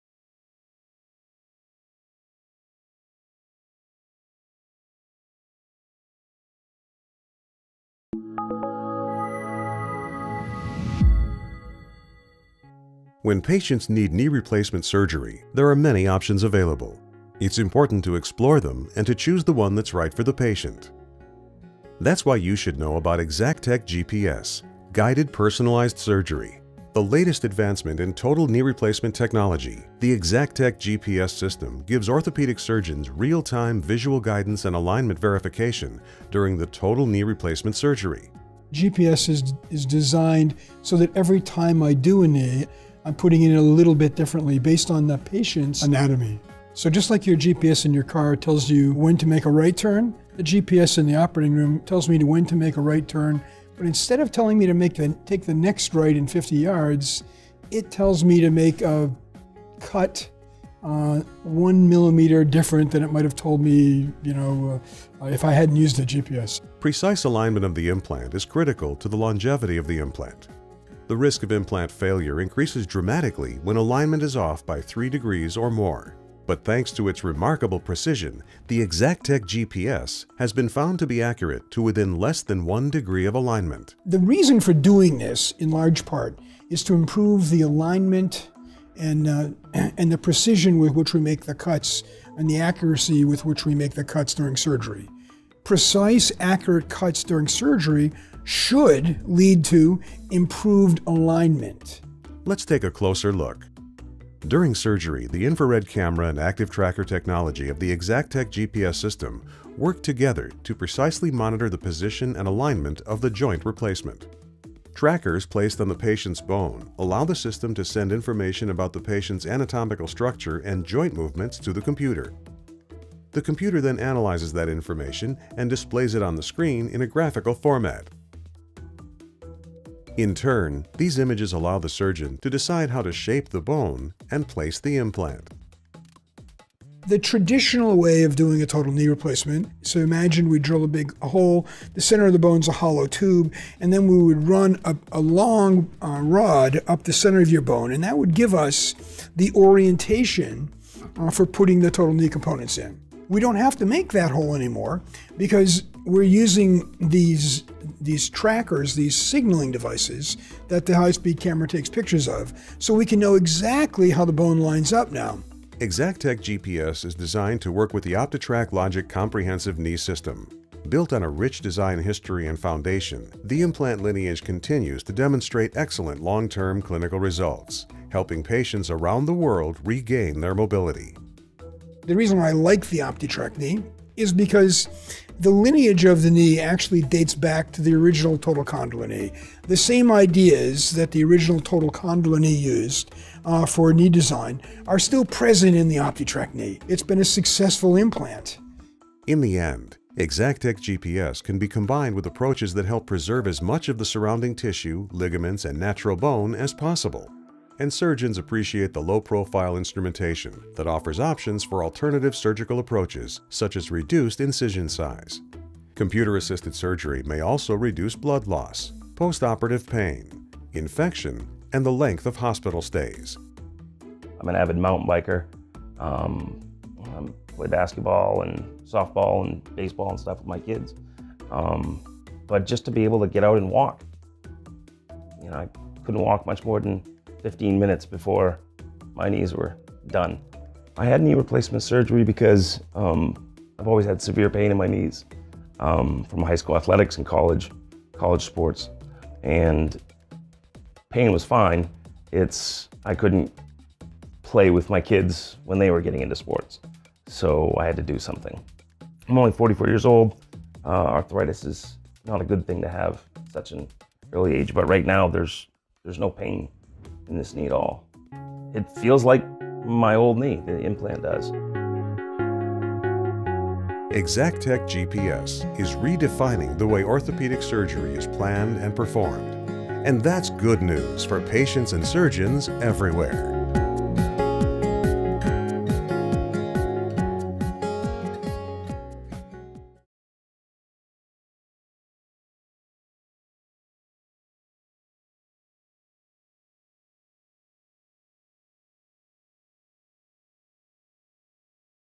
ExactechGPS at St Marys Center for Orthopaedics
Precision & Accuracy with ExactechGPS®
The latest advancement in technology for your total joint replacement surgery